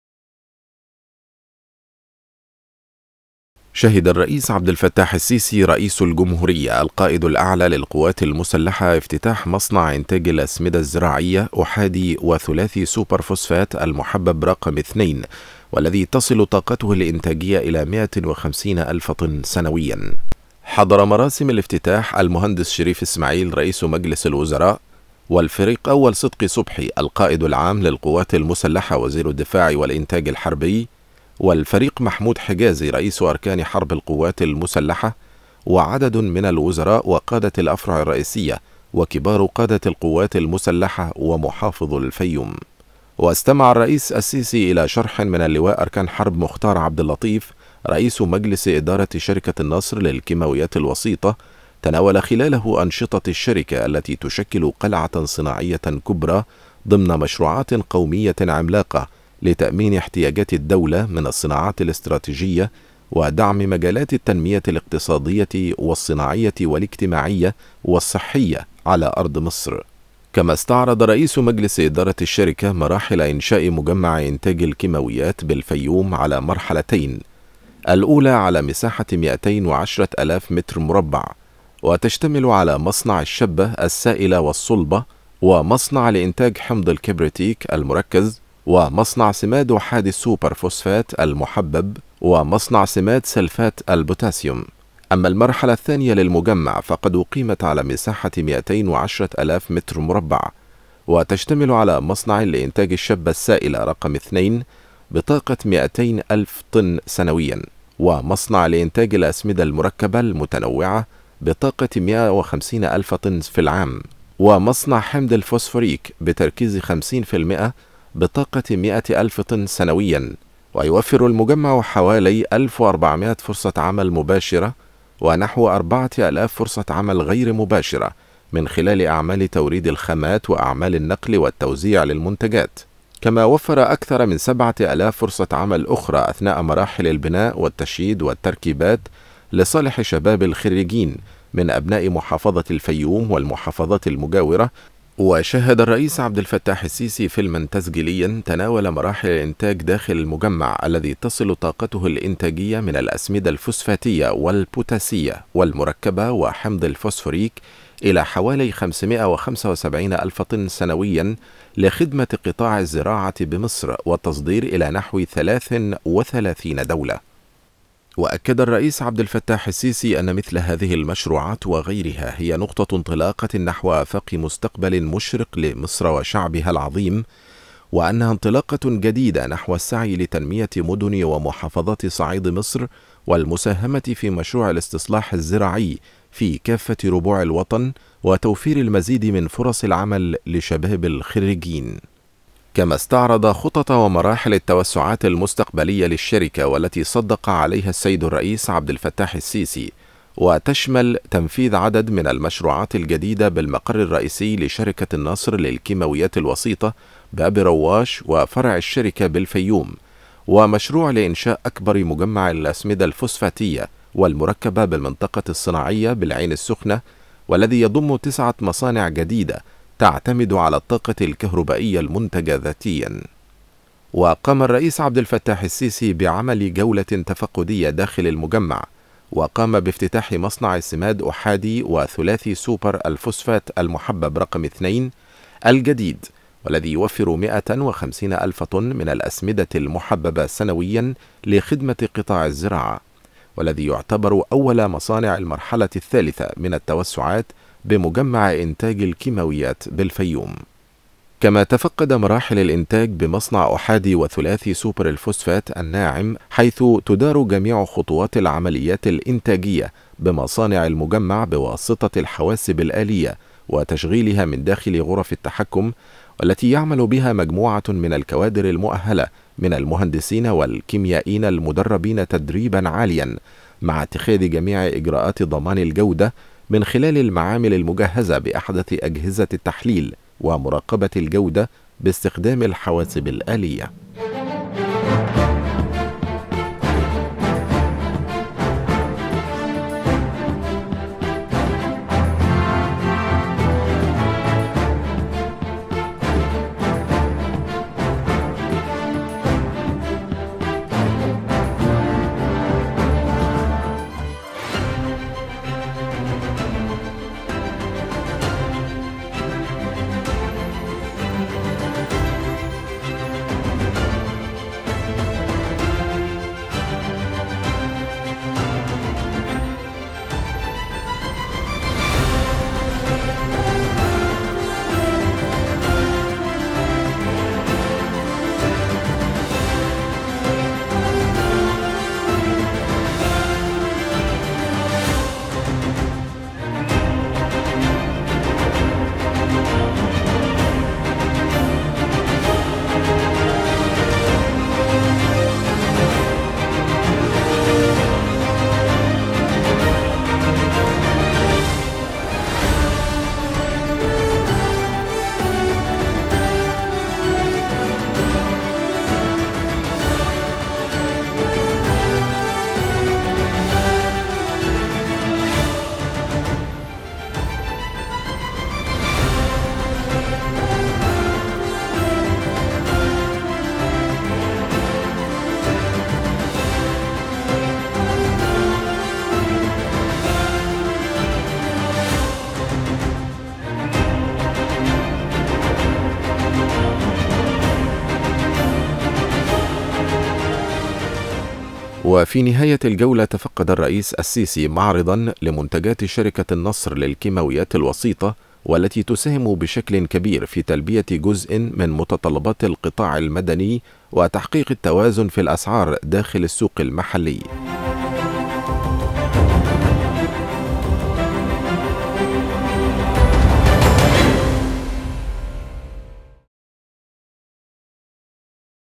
الرئيس عبدالفتاح السيسى يفتتح مصنعًا لإنتاج الأسمدة أحادى وثلاثى سوبر فوسفات المحبب
الموقع الرسمى لوزارة الدفاع المصرية : افتتح الرئيس عبدالفتاح السيسى رئيس الجمهورية القائد الأعلى للقوات المسلحة مصنع إنتاج الأسمدة الزراعية أحادى وثلاثى سوبر فوسفات المحبب رقم (2)، والذى تصل طاقته الإنتاجية إلى 150 ألف طن سنويًا، كما تفقد عددًا من الخطوط الإنتاجية الجديدة بمجمع إنتاج الكيماويات التابع لشركة النصر للكيماويات الوسيطة بالمنطقة الصناعية بكوم أوشيم بمحافظة الفيوم .